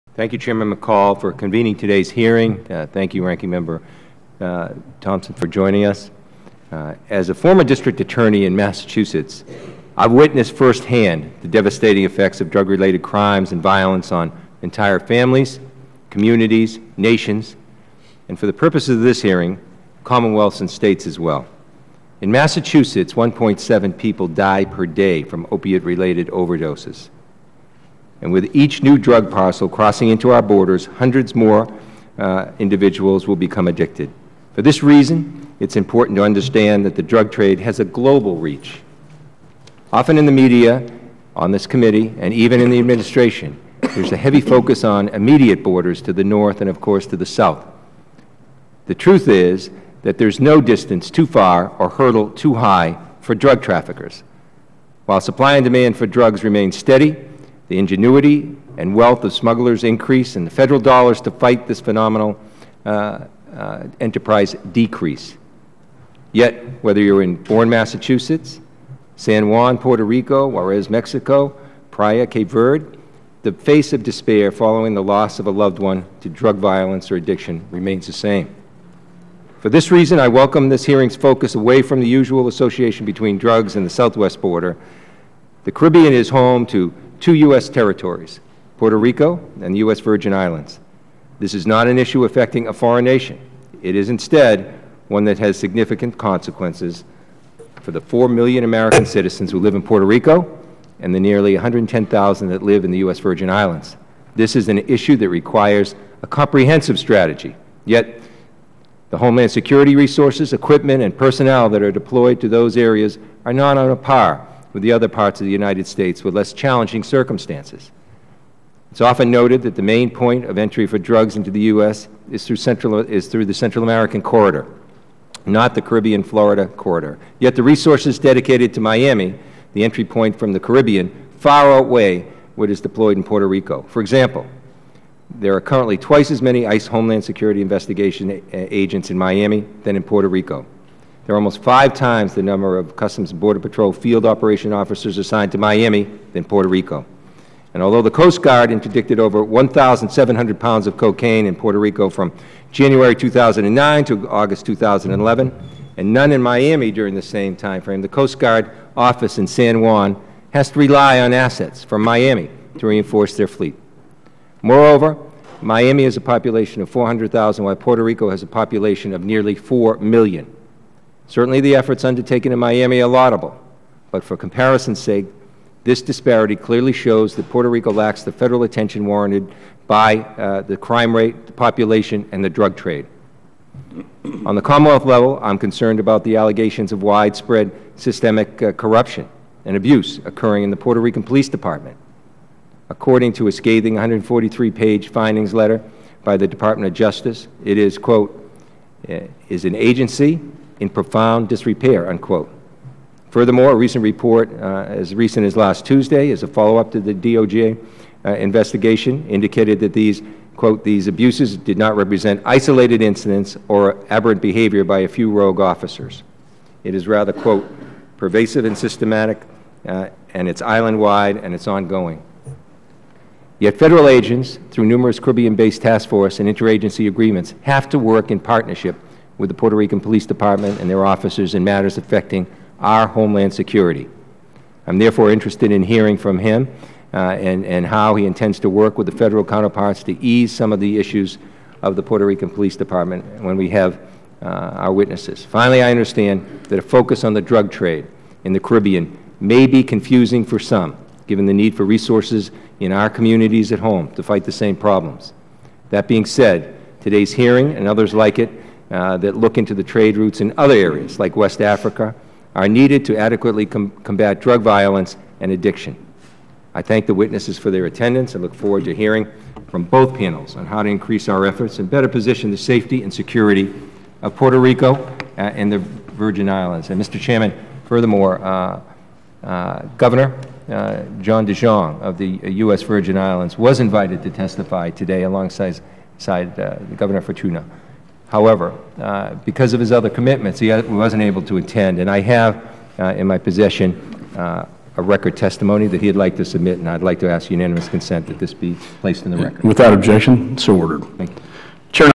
Congressman Keating -- U.S.-Caribbean Border
Rep. William R. Keating (D-MA), Ranking Member of the Committee on Homeland Security's Subcommittee on Oversight, Investigations, and Management, delivers his opening statement for the hearing on: U.S.-Caribbean Border: Open Road for Drug Traffickers and Terrorists -- June 21, 2012